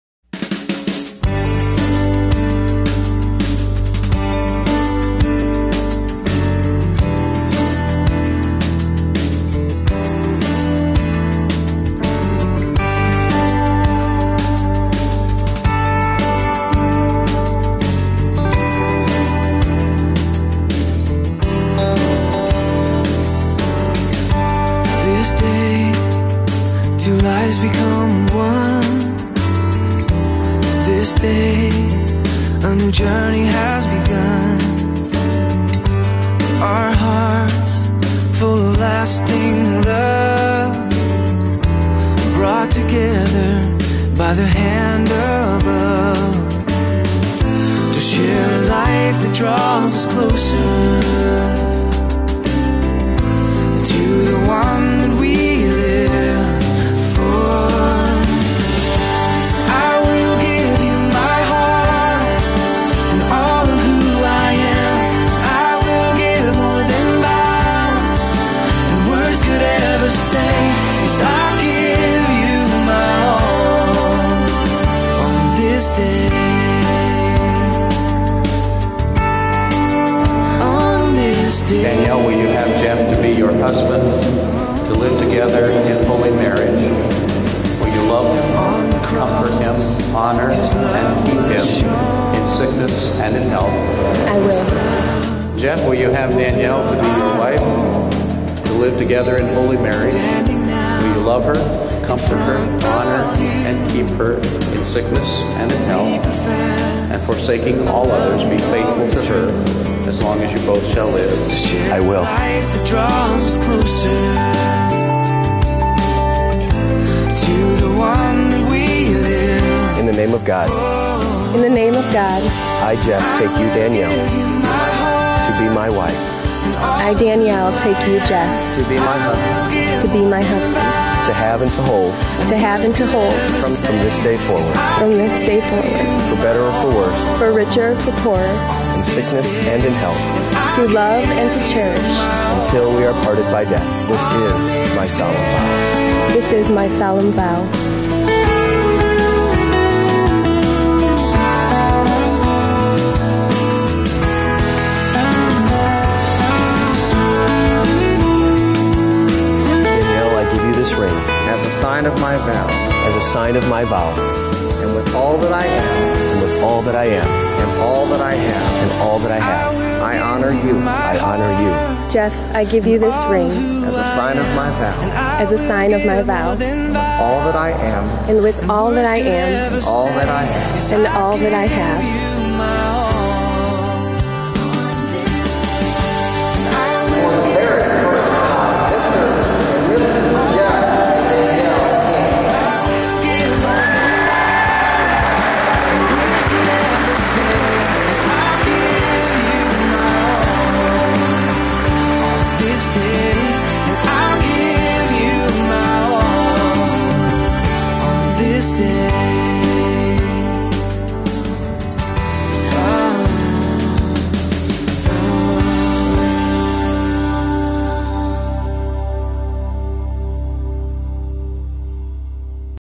Indiana Wedding Videography by Memories Custom Video, Danielle & Jeff's Wedding Highlight
Danielle & Jeff's wedding highlight from thier wedding on June 27, 2009. They got married at the Valpo University Chapel.